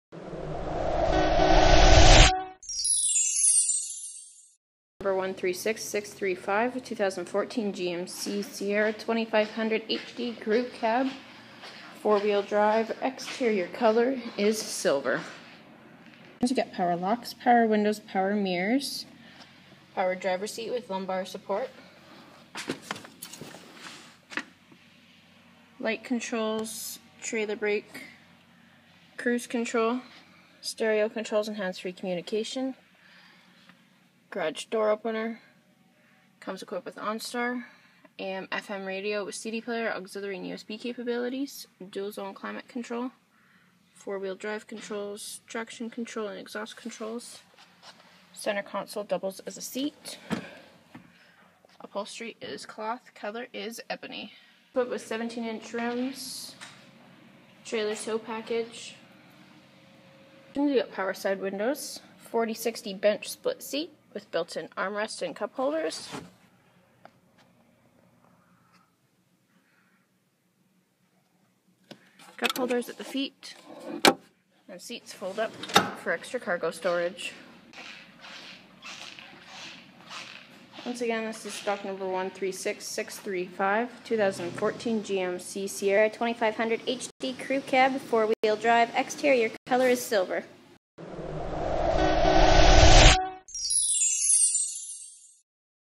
2014 GMC Sierra 2500HD 4WD Crew Cab 153.7" SLE
This is a 2014 GMC Sierra 2500HD 4WD Crew Cab 153.7&quot; SLE with 6-Speed A/T transmission Silver[GAN,Quicksilver Metallic] color and Ebony interior color.  This video is recorded and uploaded by cDemo Mobile Inspector.                             Detailed vehicle info with more photo(s) is available